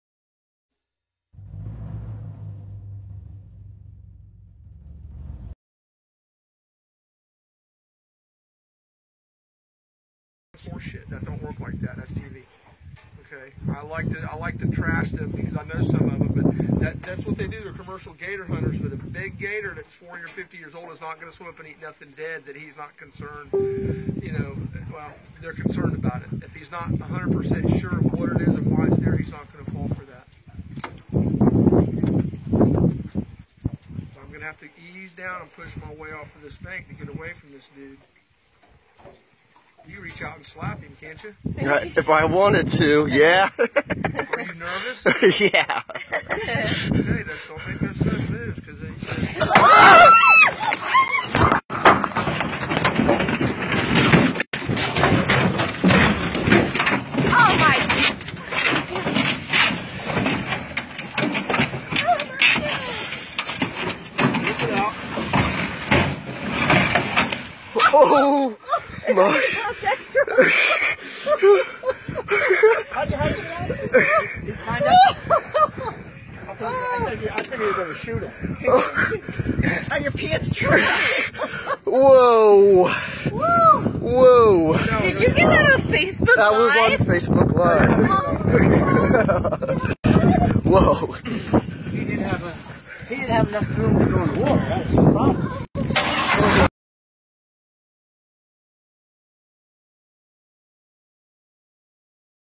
Alligator Attacks Swamp Tour Group *WARNING not GRAPHIC*
This could have been a bad situation. What do you think? Would you go again?